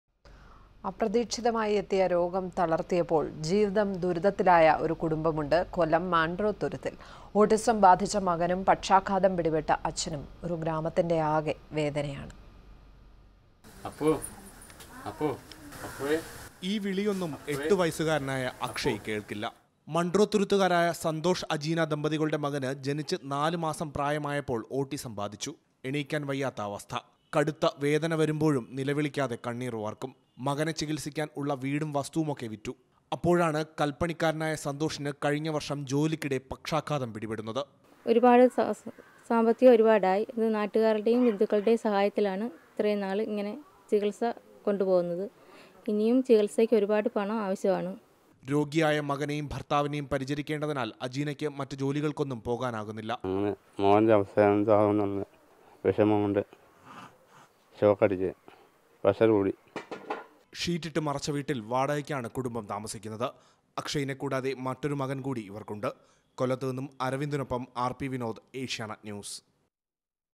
Poor Father and Kid seek financial aid For Treatment
Autism affected kid and stroke affected father seeks financial aid for Treatment